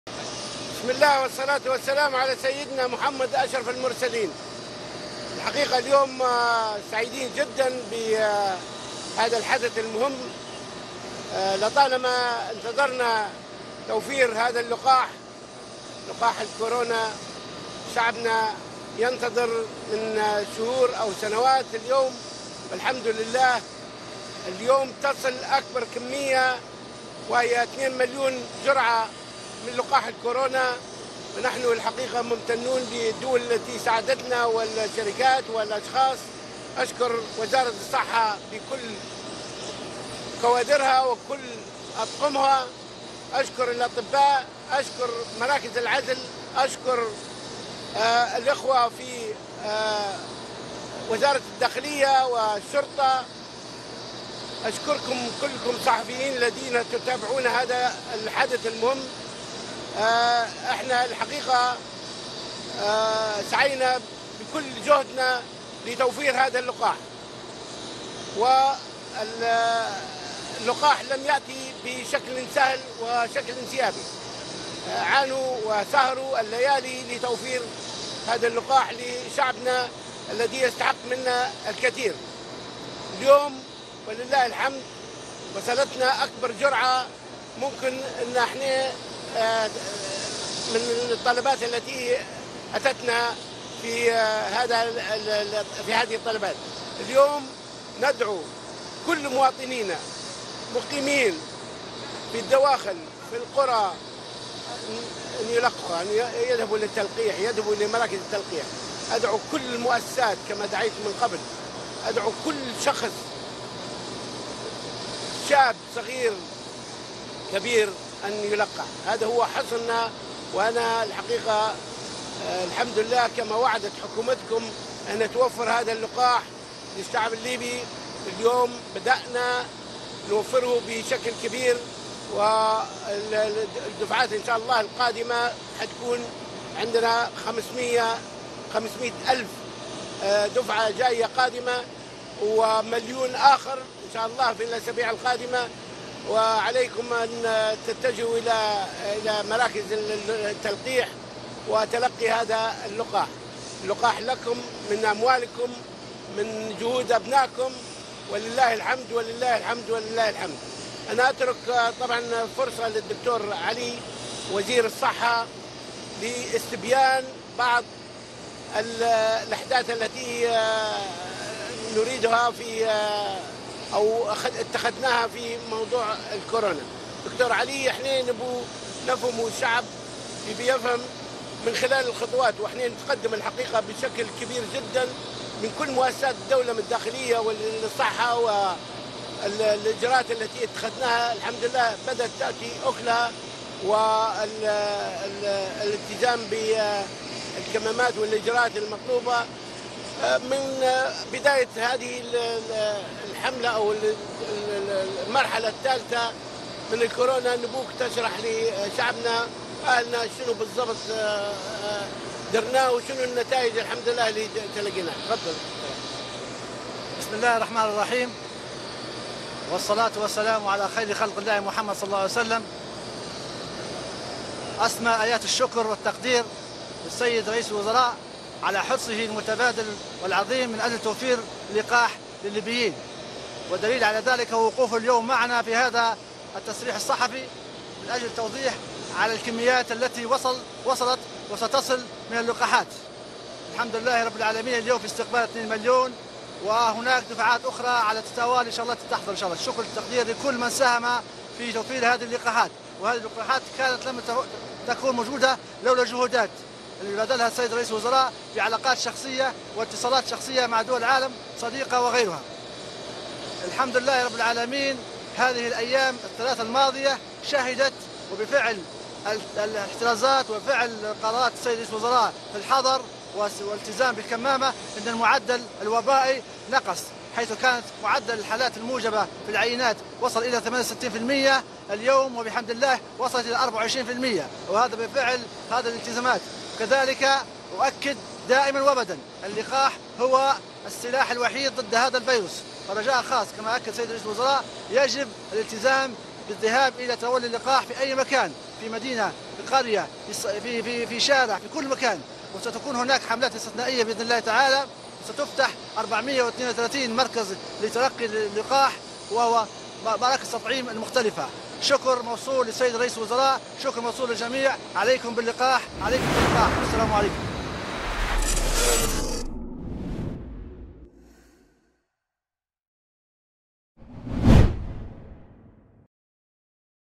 الدبيبة: مليون وخمسمائة ألف جرعة ستصل خلال الأسابيع القادمة
شاهد | الدبيبة: مليون وخمسمائة ألف جرعة ستصل خلال الأسابيع القادمة

ــــــــــــــــــــــــــــــــــــــــــــــــــــــــــــــــــــــــــ
ــــــــــــــــــــــــــــــــــــــــــــــــــــــــــــــــــــــــــ
يمكنكم تحميل تطبيقنا TANASUH TV على الهواتف الذكية: